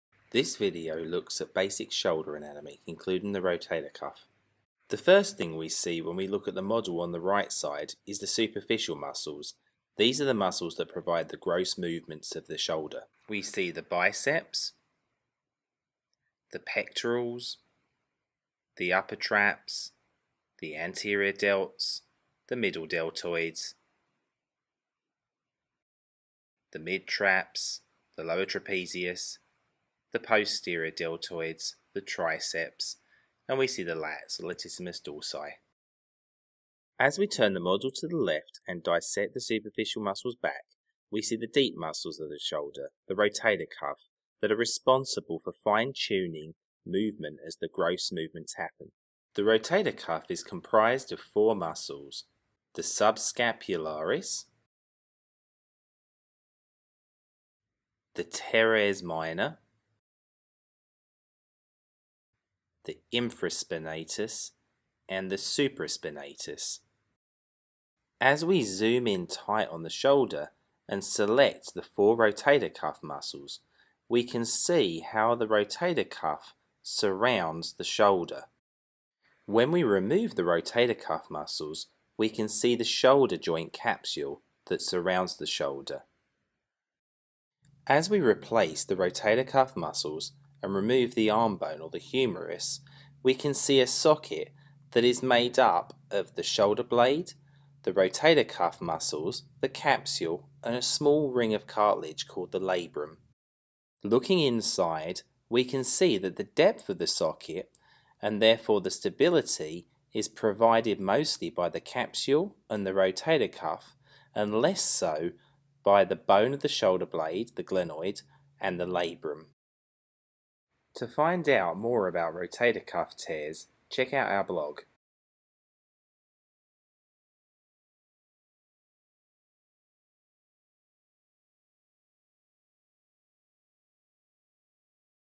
Rotator Cuff Anatomy
Rotator Cuff Tears are a common cause of shoulder pain, but are also present in people without pain. If you have shoulder pain, the rotator cuff is an extrememly important set of 4 muscles/tendons. Many think of it as one muscle, but it is made of the supraspinatus, infraspinatus, teres minor and subscapularis. If you have a tear you may not need surgery and may be able to get full recovery with physical therapy.